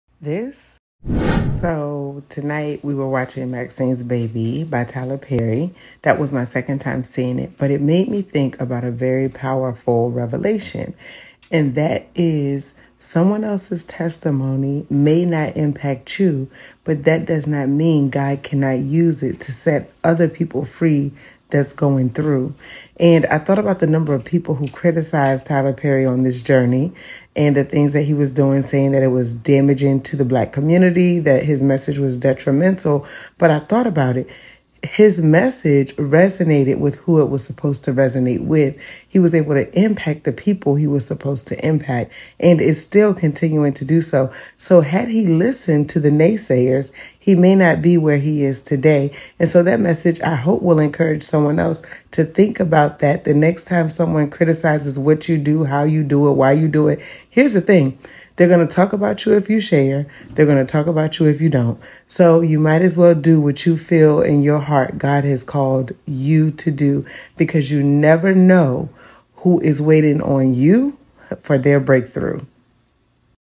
Revelation & Reflection: How Maxine's Baby by Tyler Perry Will Set Someone Free Through Testimony
🙌🏾 Someone is waiting on you for their breakthrough!!!

🗣️The Power of Testimony! Revelation & Reflection - How 'Maxine's Baby' by Tyler Perry is a Blessing

🎬 I watched Maxine's Baby by Tyler Perry a 2nd time last night and it revealed a powerful lesson. Even if someone's testimony doesn't resonate with us, God can use it to touch and change the lives of others. Let's not discredit the experiences of others, but instead, let's learn and grow from them.

📖 “They triumphed over him by the blood of the Lamb and by the word of their testimony; they did not love their lives so much as to shrink from death.” Revelation‬ ‭12:11‬ ‭NIV‬‬